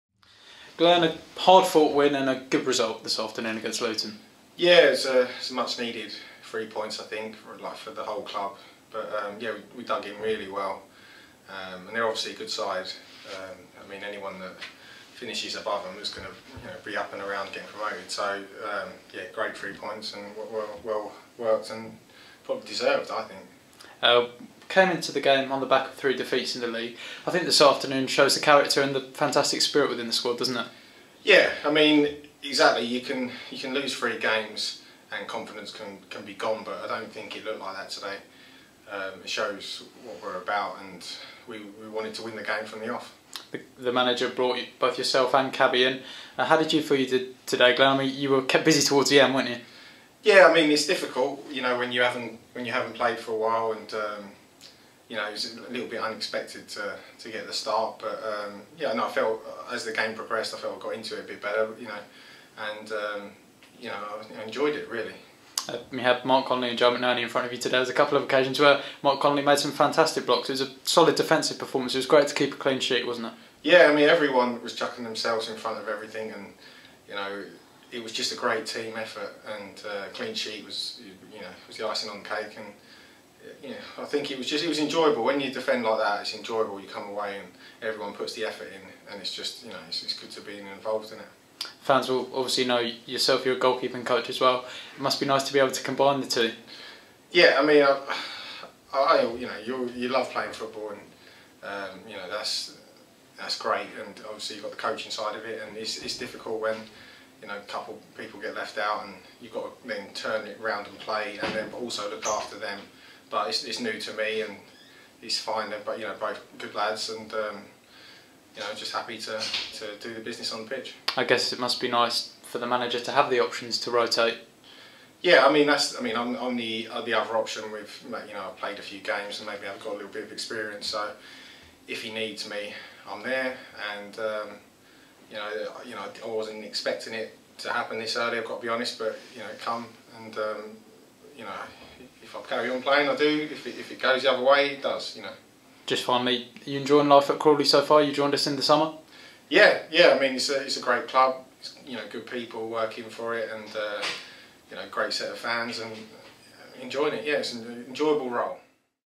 Glenn Morris on winning debut against Luton Town
Hear from goalkeeper Glenn Morris after he made his debut and  for the club and kept a clean sheet in the 2-0 win over Luton Town on Saturday.